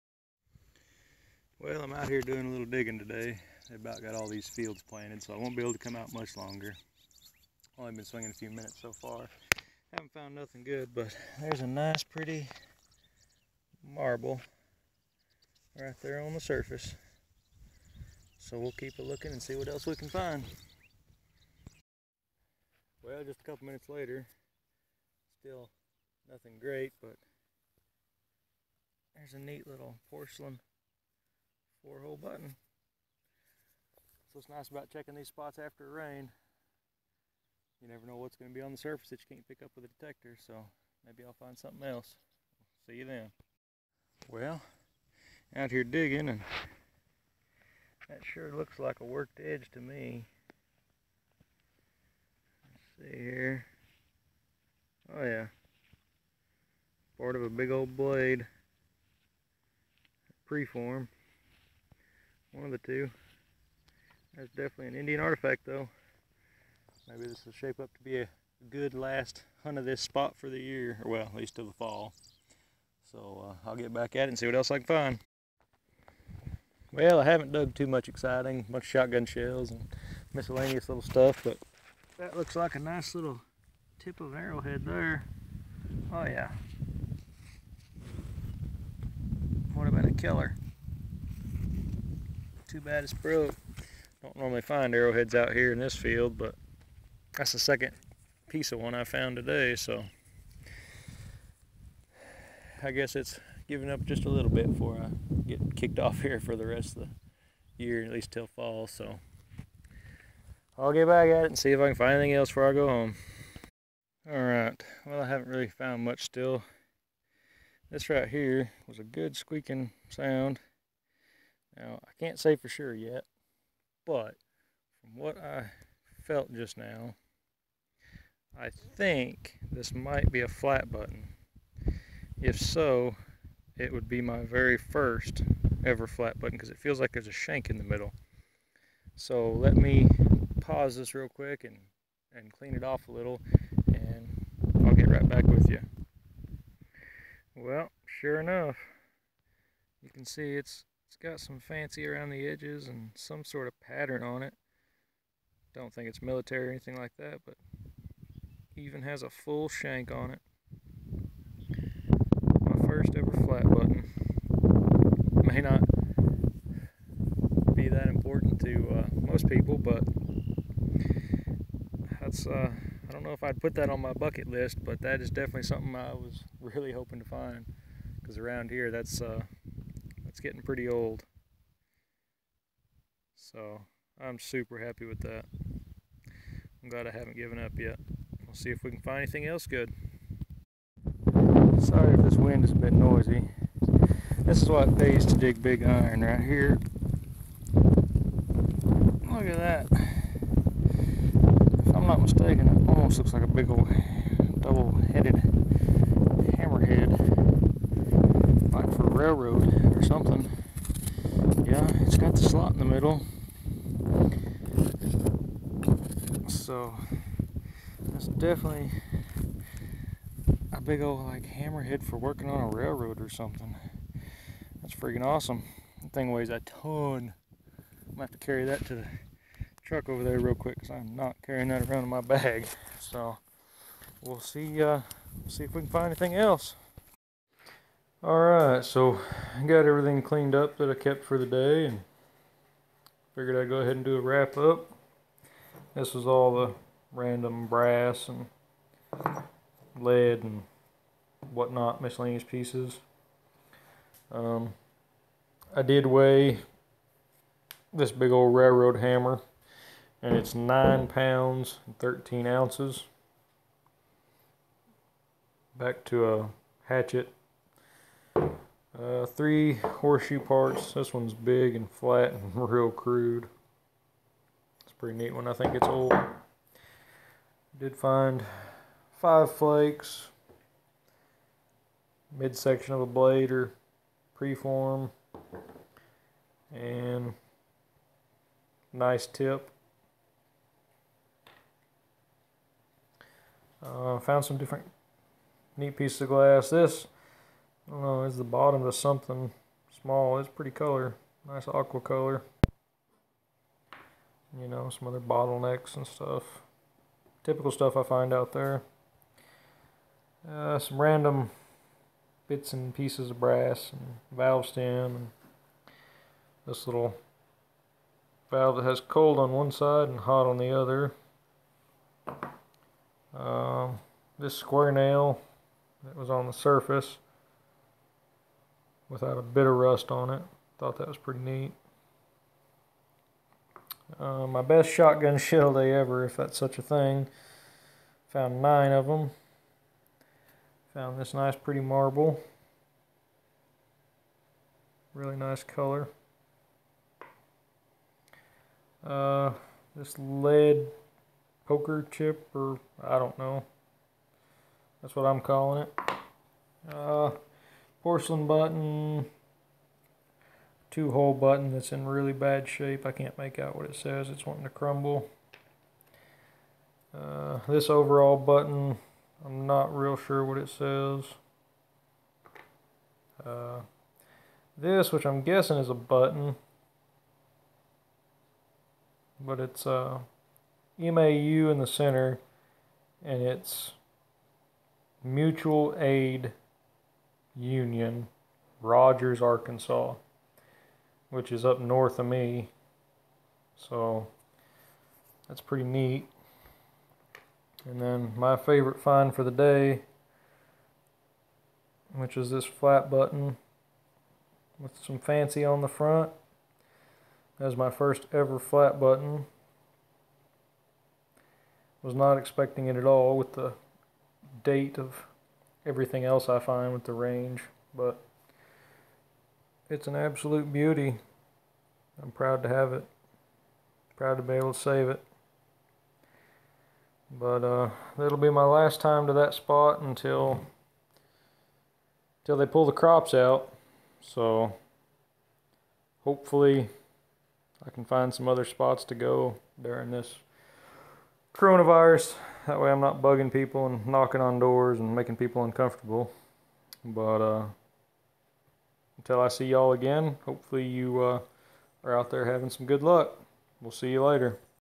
Last hunt till the crops are pulled for the honey hole.
Last hunt of the spring in the farm feilds cause crops going in. Arkansas digging. History.